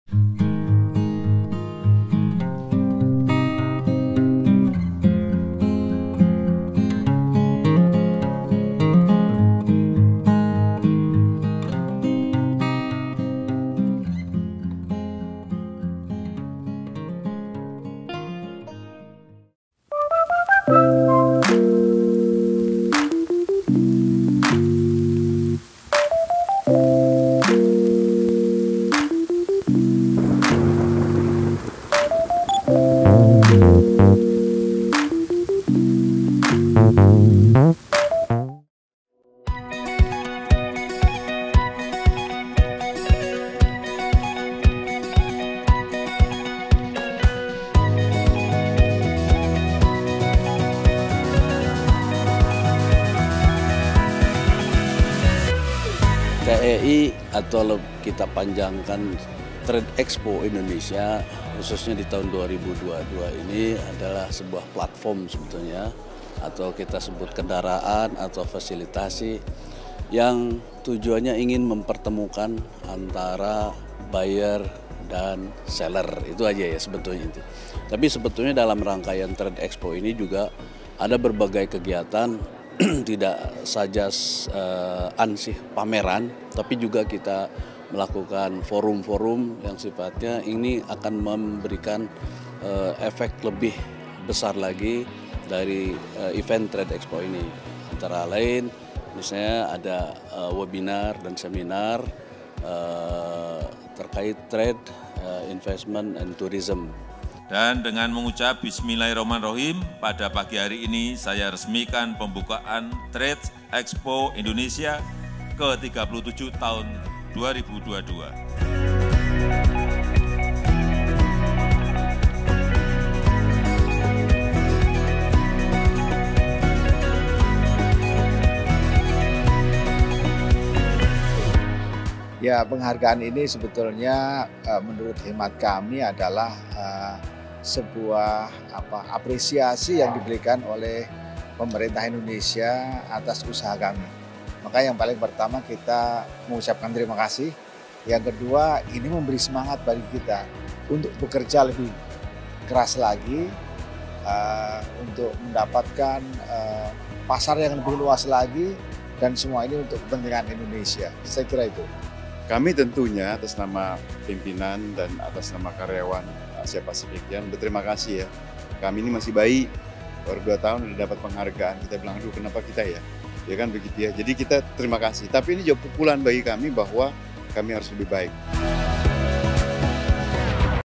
Buka Expo Indonesia TEI, Jokowi Diadang Peserta untuk Foto Bareng, Presiden Tersenyum Bangga
Buka Expo Indonesia TEI, Jokowi Dihadang Peserta untuk Foto Bareng, Presiden Tersenyum Bangga